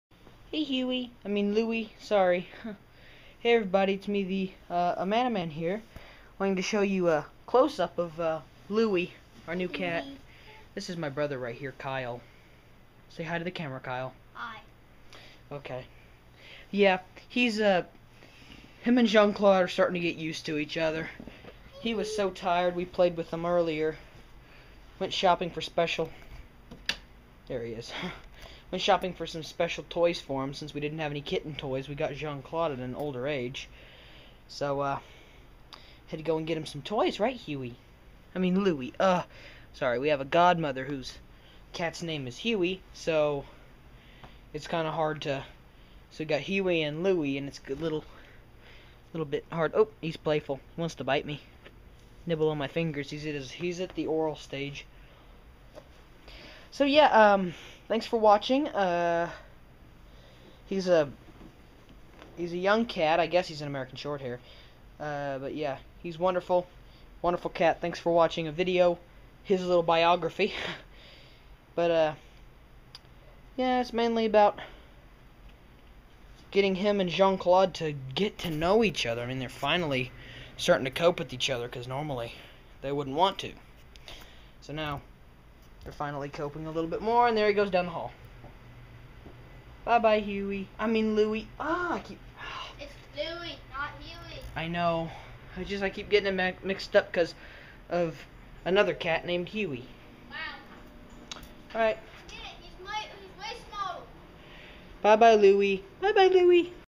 Talking about our new cat Louie!
Me and my brother Kyle talking about our new American Shorthair cat, Louie!